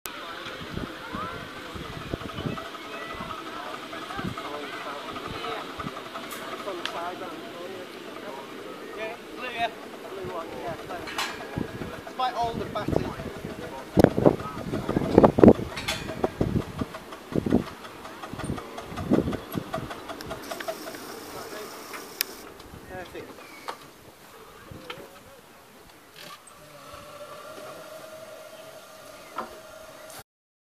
C.&.D.M.E.S at Hammond's Pond: The 7¼ inch Gauge Live Steam is, Sweet William 0-4-0ST was arriving.
The Carlisle And District Model Engineering Society at Hammonds Pond in City of Carlisle: The Seven an Quarter (7 1/4) inch Gauge his Live Steam Locomotive was The ‘Sweet William’ (Four-Wheel-Switcher) 0-4-0ST of a NARROW GAUGE, as New Ownership Big Loco by Tim MacDonald in Club Member of Live Steam, so was on The Passenger Train onto Running on the C.&.D.M.E.S Outdoor Track on Sunday 14th May 2023